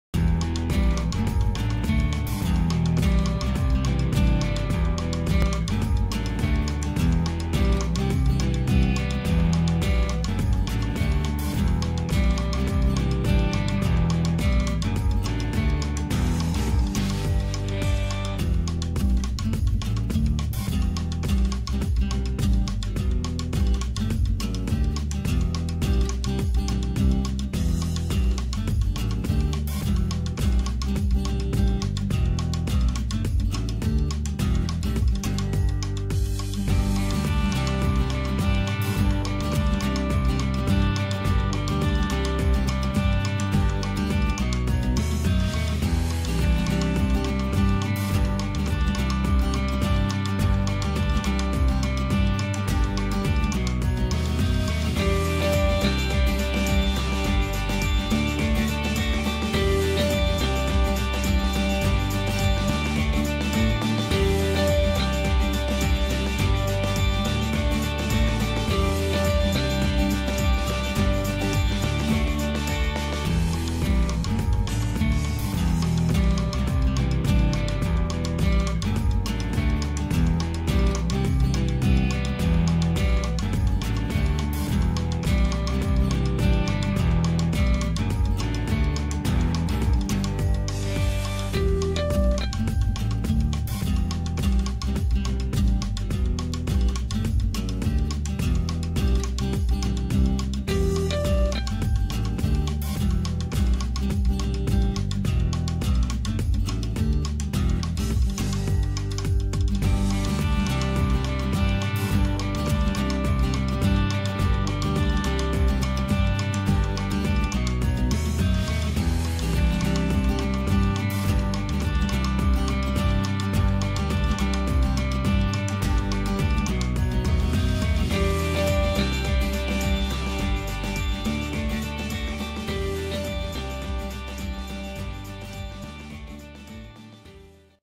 360 VIEW Funko Pop First Order Executioner Bib Fortuna Guavian Plaetorian Guard
First Order Executioner No. 201
Plaetorian Guard No. 200
Bib Fortuna No.53
GuavianNo.112